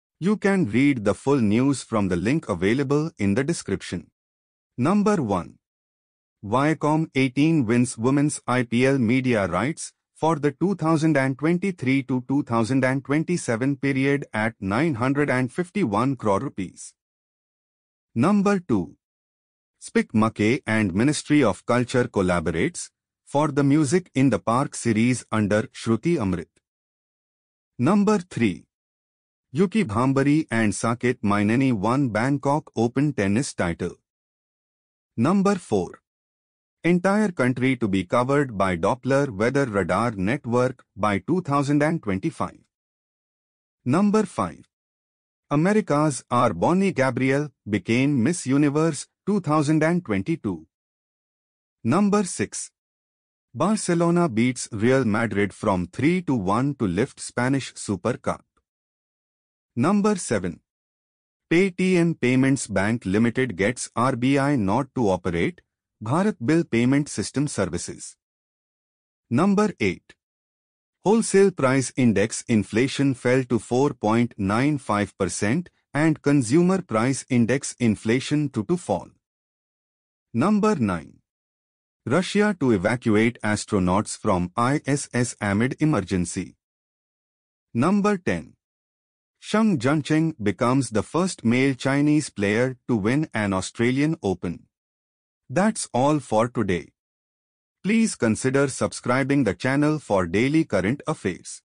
17 Jan Daily Current Affairs : 16 January 2023 | Triumph IAS Daily News & Views
For Full news Analysis Open the Link:

News Overview
1. Viacom18 wins Women's IPL media rights for the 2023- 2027 period at INR 951 cr.
2. SPIC MACAY and Ministry of Culture collaborates for the 'Music in the Park' series under "Shruti Amrut".
3. Yuki Bhambri and Saketh Myneni won Bangkok Open tennis title.
4. Entire country to be covered by doppler weather radar network by 2025.
5. America's R Bonnie Gabriel became Miss Universe 2022.
6. Barcelona beats Real Madrid 3-1 to lift Spanish Super Cup.
7. Paytm Payments Bank Limited gets RBI nod to operate Bharat Bill Payment System services.
8. Wholesale price index inflation fell to 4.95% , and CPI inflation too to fall (Ld).
9. Russia to evacuate astronauts from ISS amid emergencey.
10. Shang Juncheng becomes the first male Chinese player to win an Australian Open.